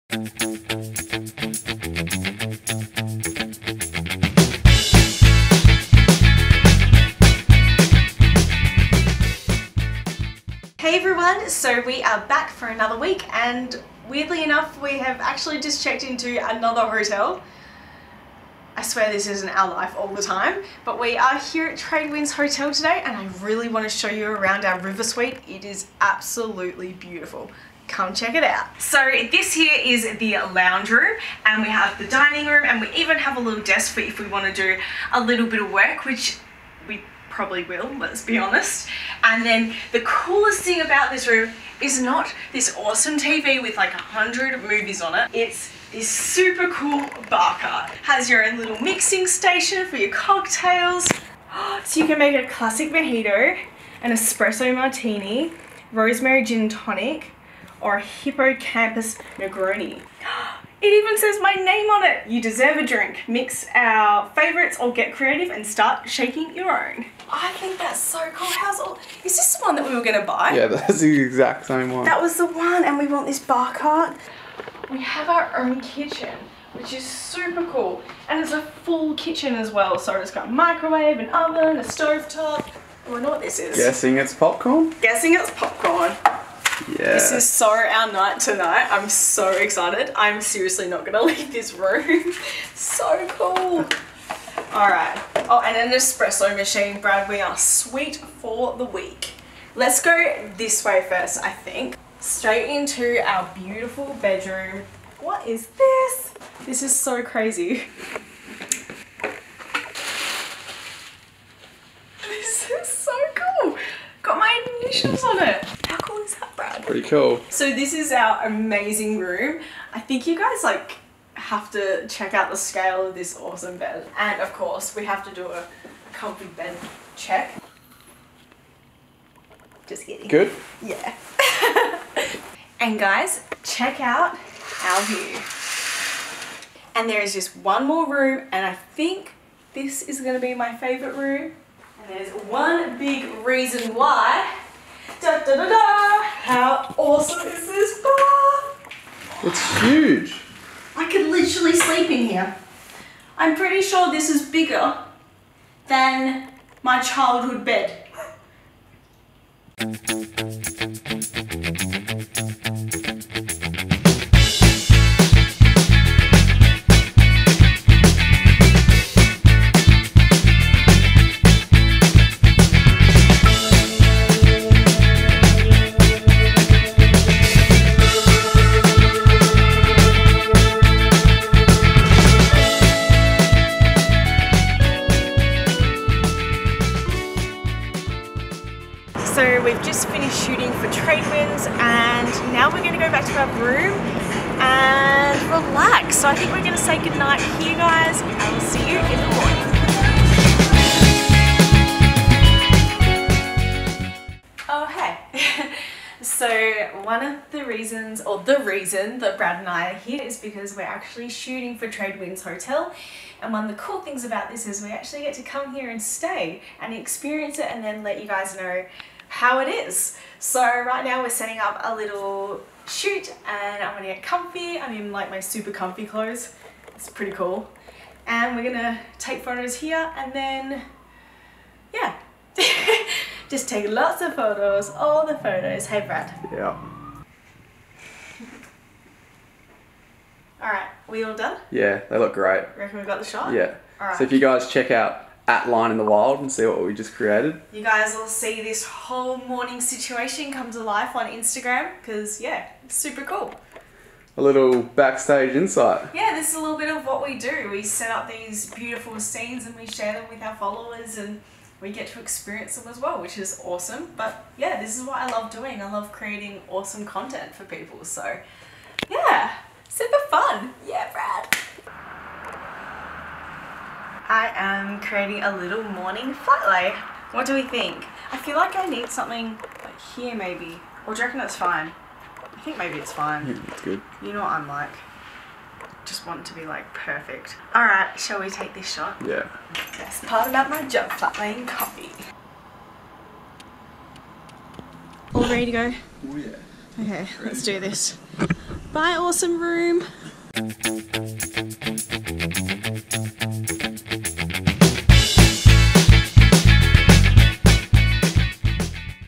THIS IS SO CRAZY! | Tradewinds Hotel | VLOG 005
Flat laying, styling and taking lots of Instagram photos at the newly refurbished and luxe Tradewinds Hotel in iconic Fremantle.